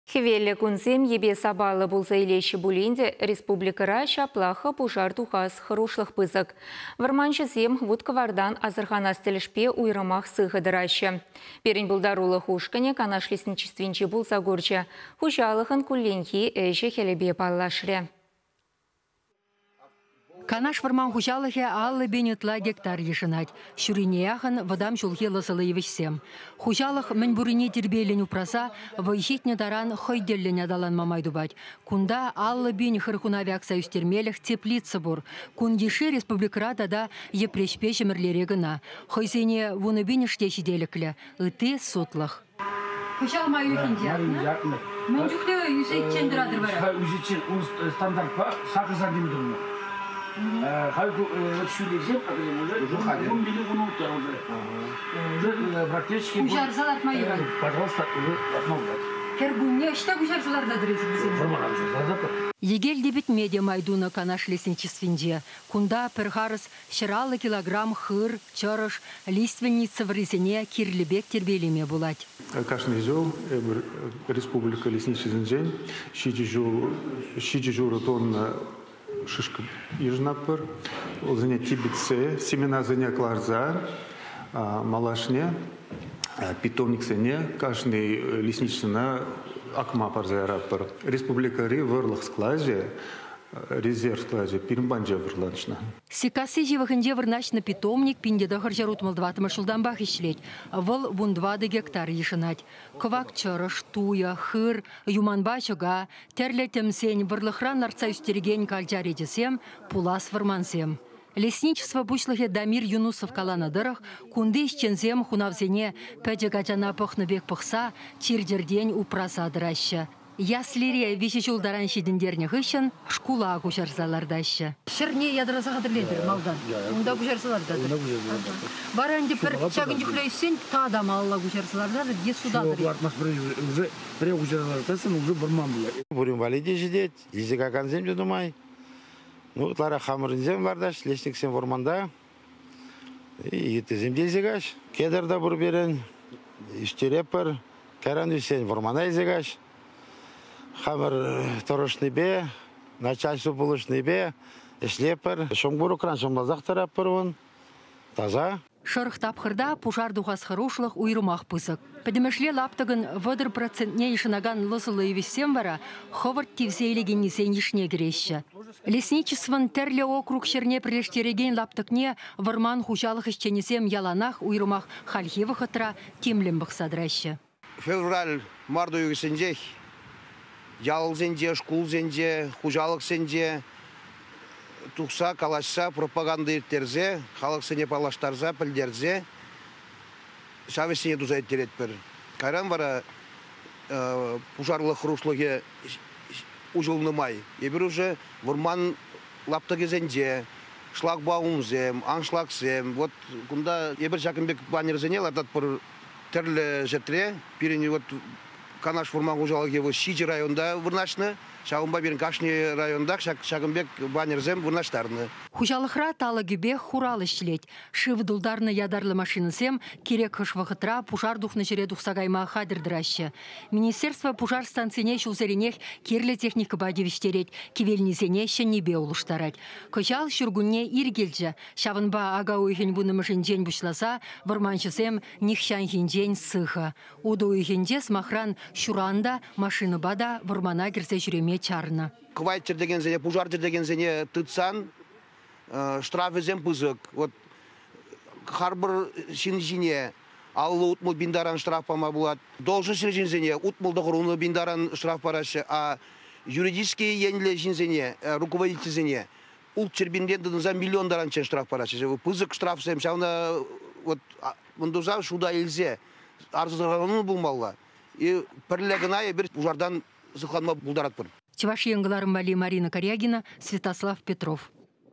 Хунавсене ачана пӑхнӑ пек пӑхаҫҫӗ
Хӗвеллӗ кунсем йӗпе-сапаллӑ пулса илеҫҫӗ пулин те, республикӑра ҫаплах-ха пушар тухас хӑрушлӑх пысӑк. Вӑрманҫӑсем вут-кӑвартан асӑрханас тӗлӗшпе уйрӑмах сыхӑ тӑраҫҫӗ. Пирӗн пултарулӑх ушкӑнӗ Канаш лесничествинче пулса курчӗ, хуҫалӑхӑн кулленхи ӗҫӗ-хӗлӗпе паллашрӗ.